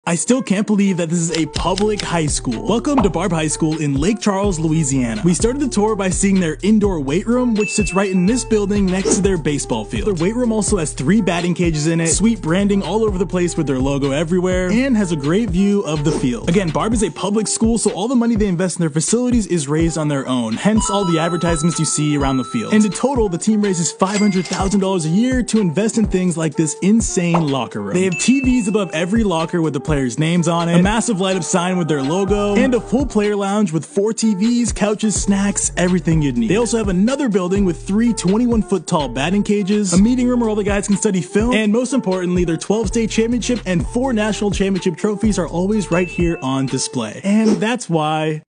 Their Baseball Facilities are UNREAL 😱🔥⚾️
No. 22 Barbe (LA) has UNBELIEVABLE facilities 😱⚾️

(🎥  DSARM )